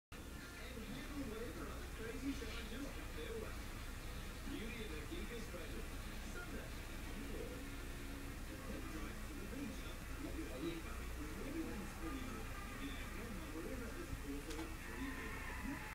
RDC introducing X_fight_me69
New guy good friend doing well just starting in his Nissan skyline